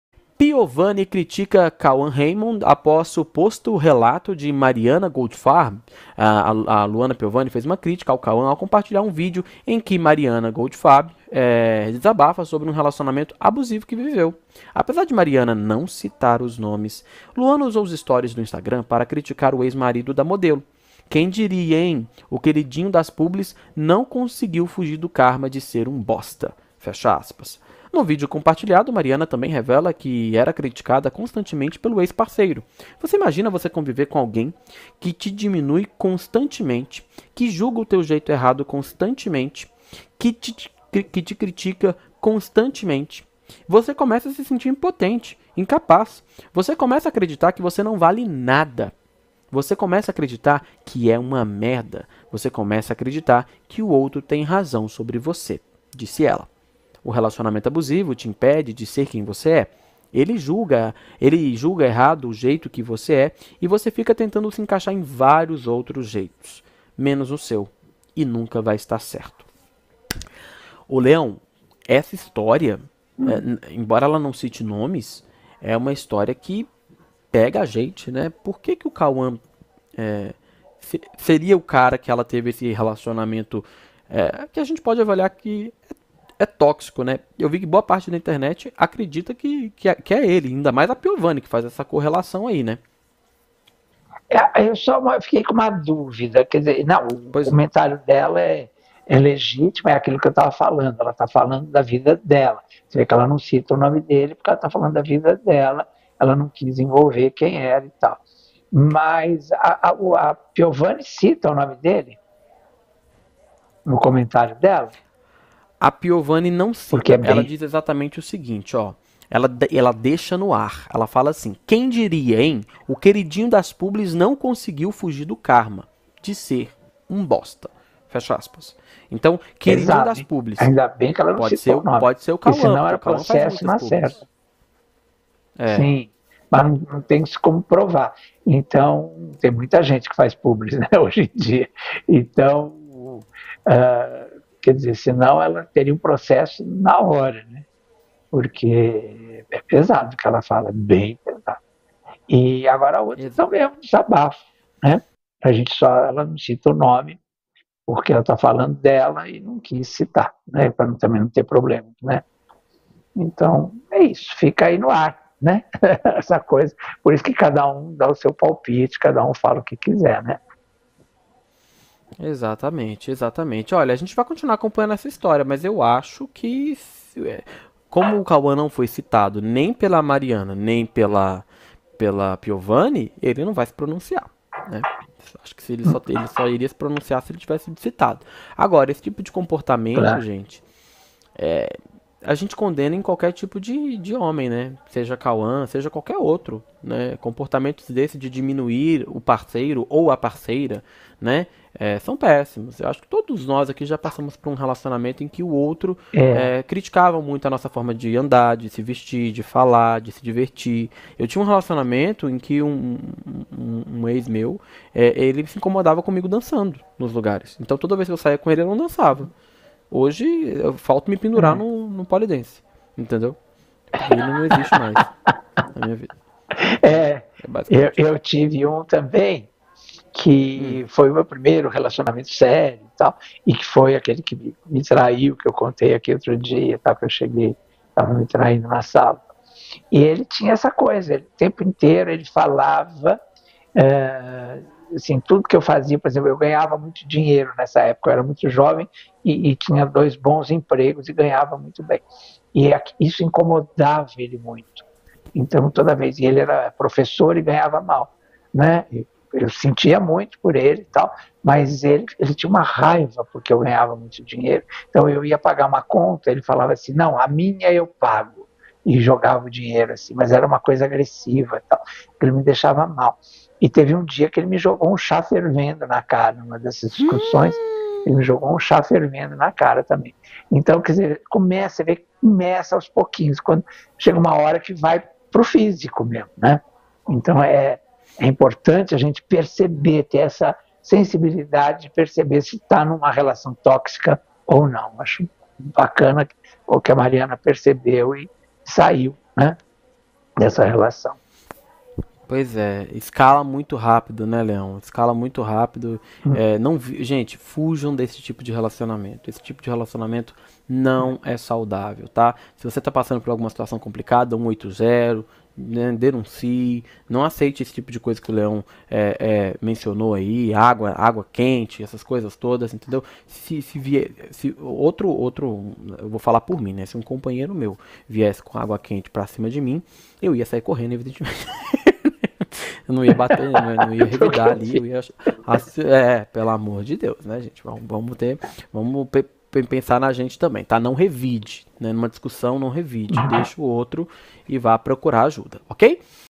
Luana Piovani critica Cauã Reymond depois de relato de Mariana Goldfarb 'Um bosta'
Luana Piovani fez uma crítica a Cauã Reymond ao compartilhar um vídeo em que Mariana Goldfarb desabafa sobre um relacionamento abusivo que viveu.